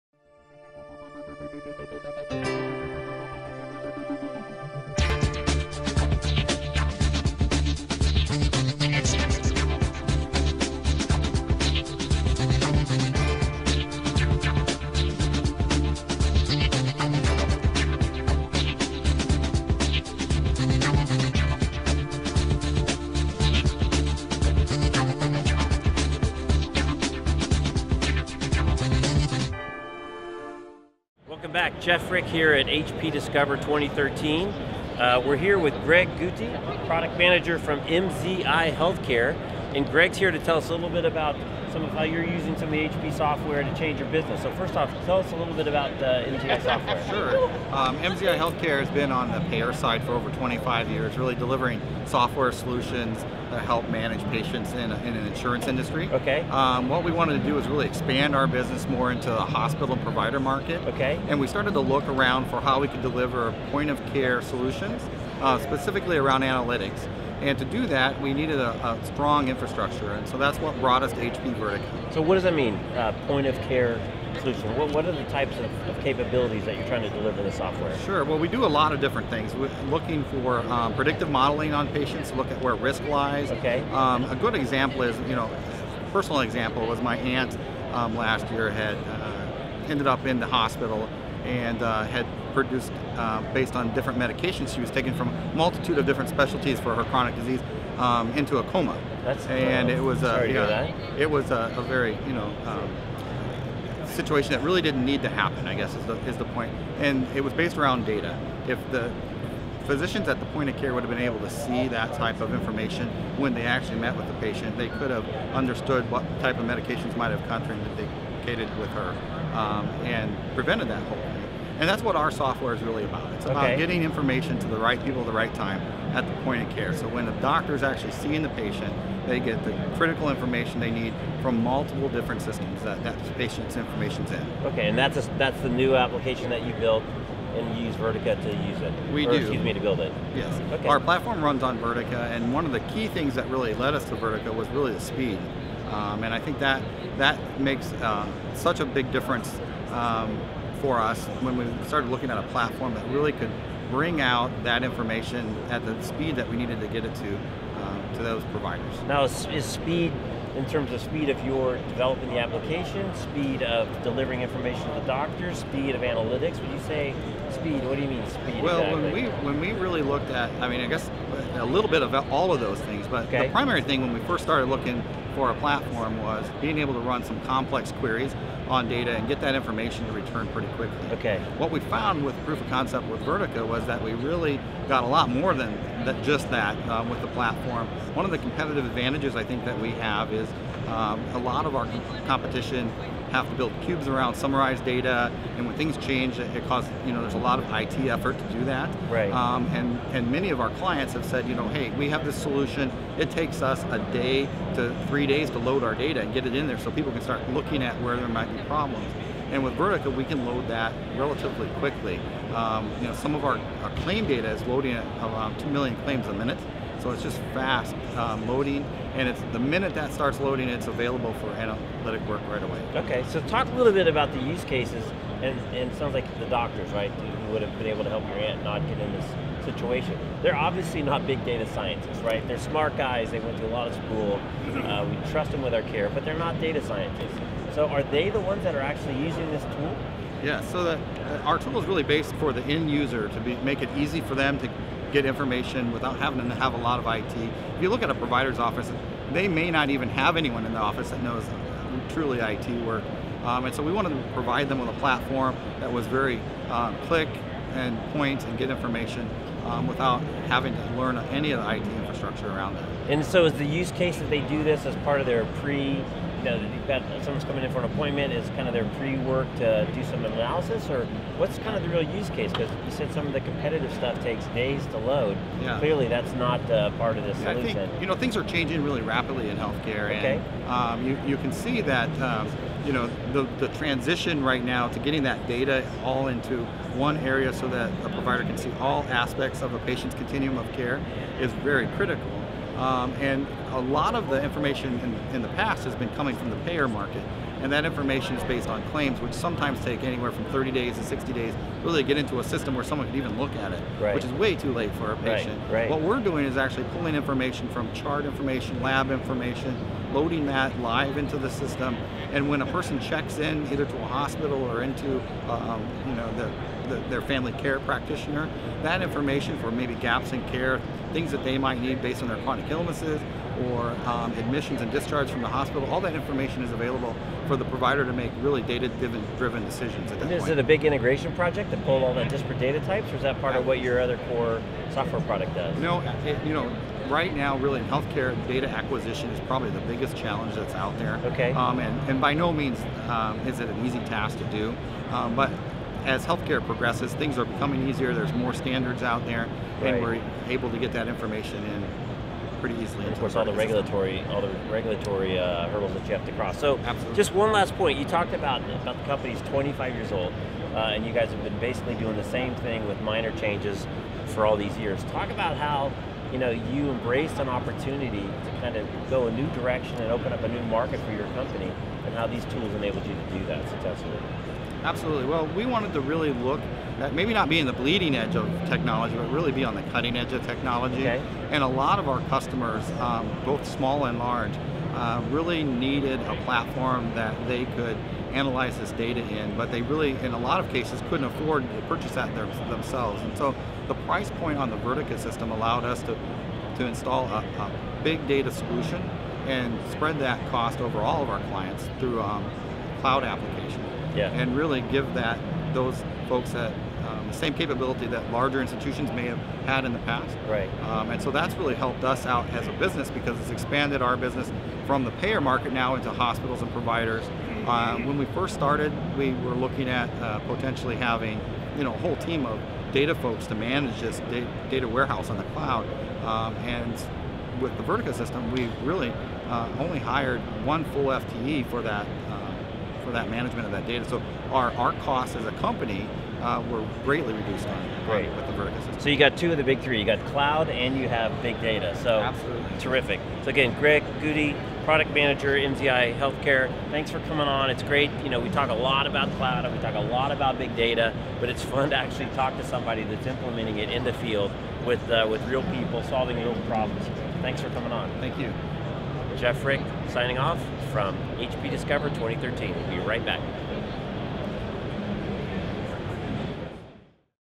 Greg Gootee, MZI Healthcare | HP Discover 2013 - Studio B
Greg Gootee, MZI Healthcare, at HP Discover 2013 with Jeff Frick